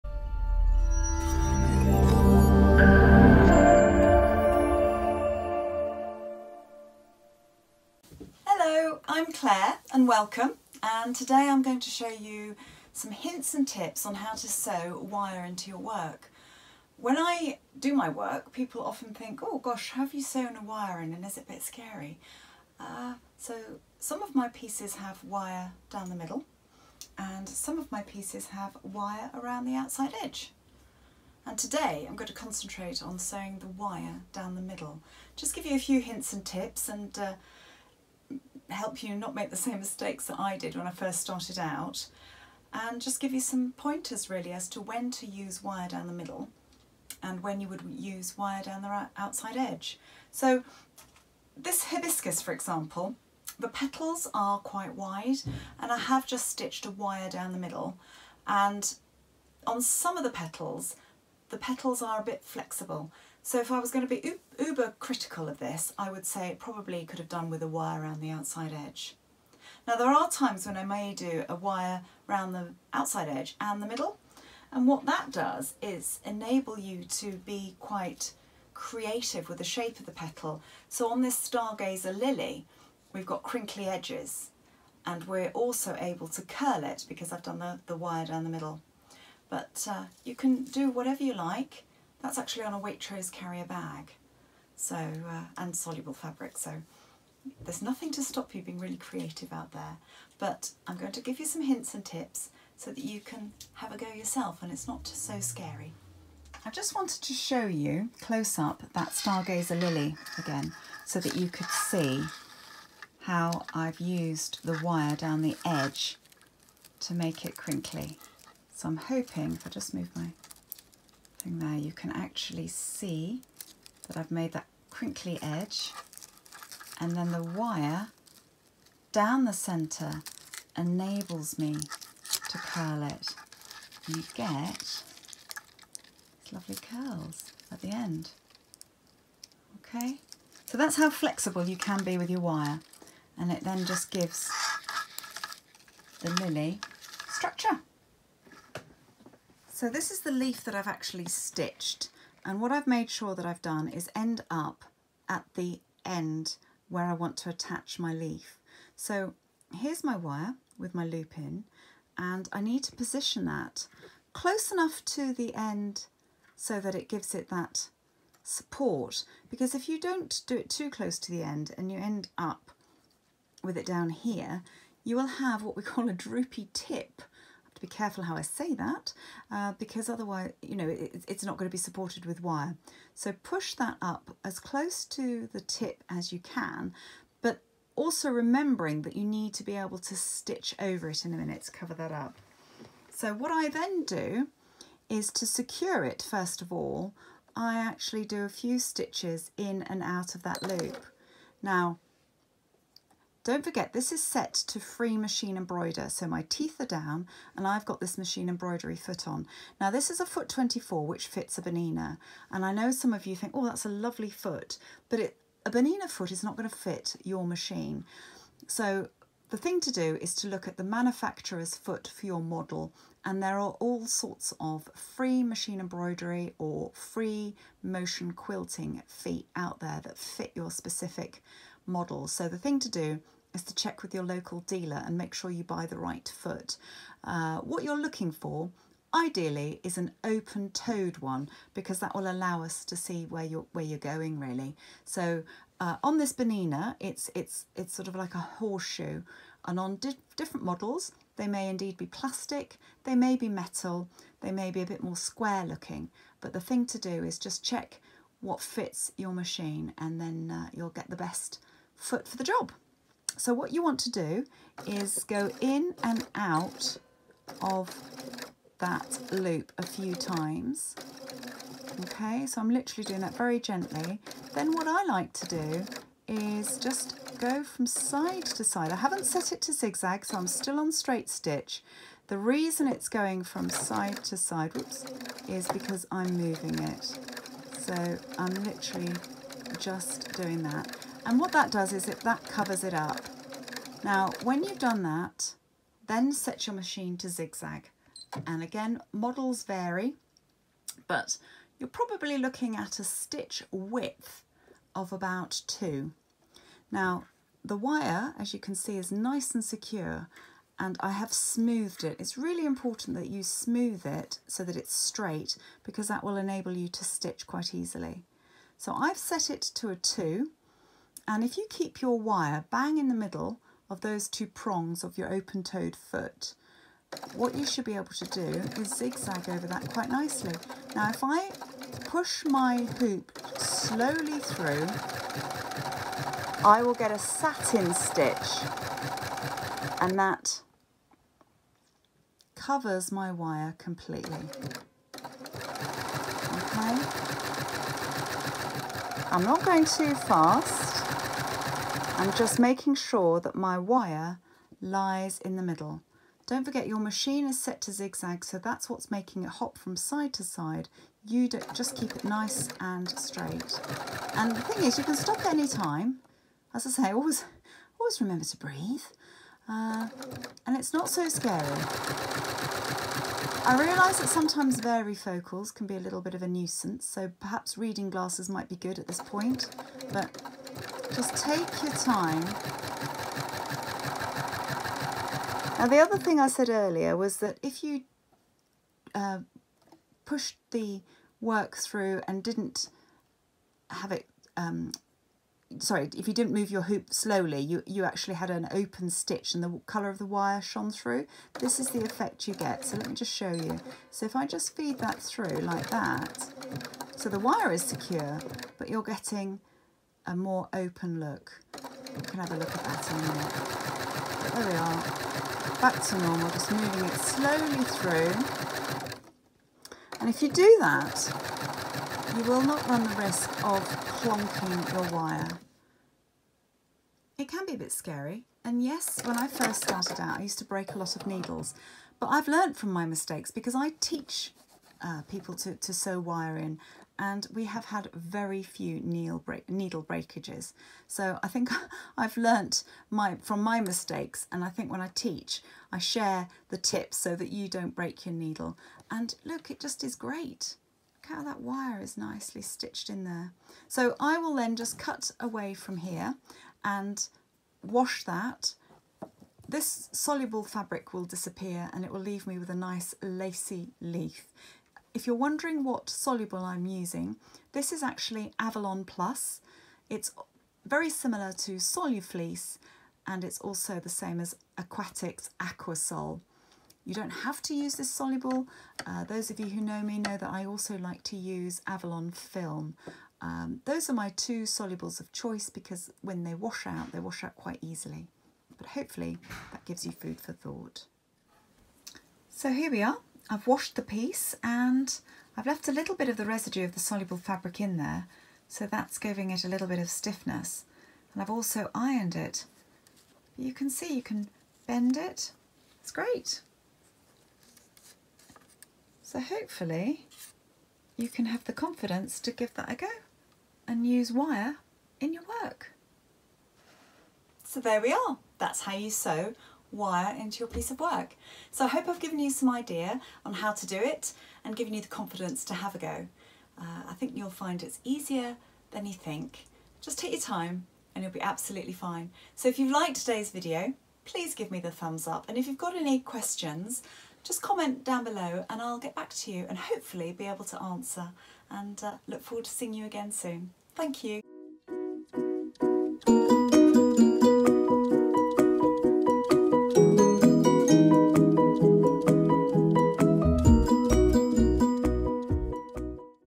Sewing over Wire
Claire shows you how to sew wire onto a free machine embroidered leaf which has been stitched on soluble fabric.